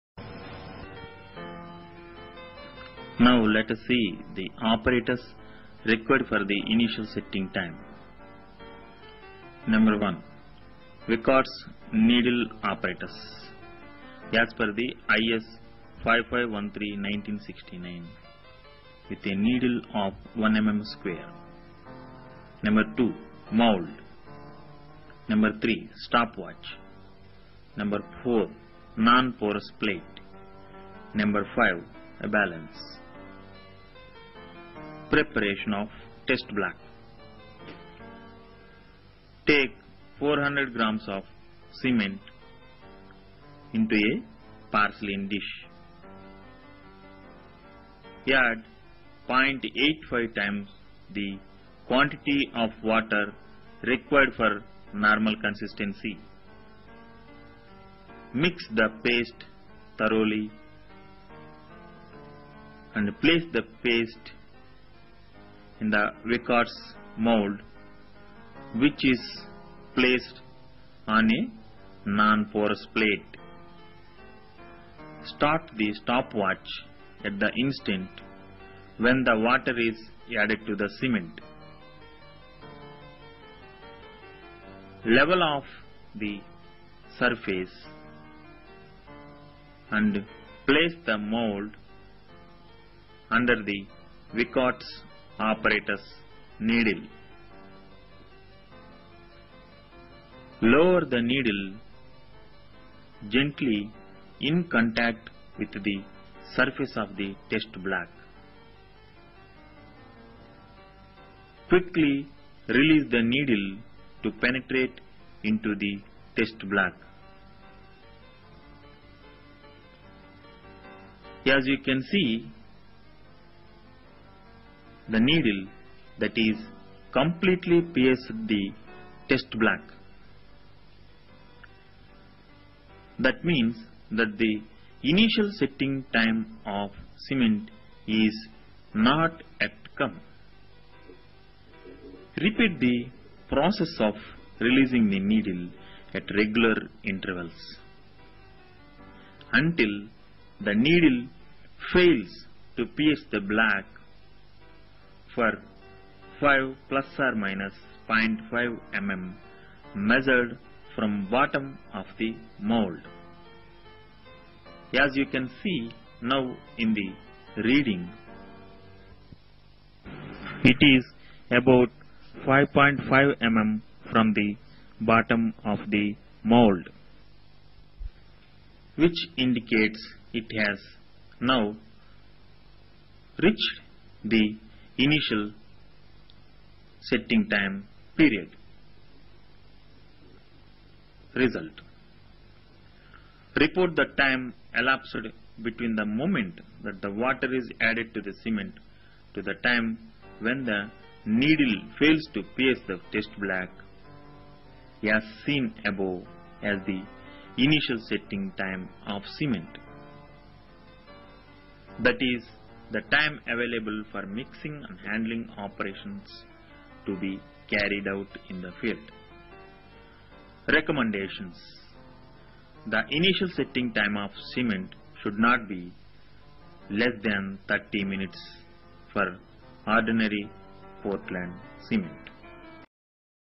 Test for Cement InitialSetting Time اختبار زمن الشك الابتدائى
يصف الفيديو اختبار زمن الشك الابتدائى والوقت الذى ياخذه الاسمنت كى يشك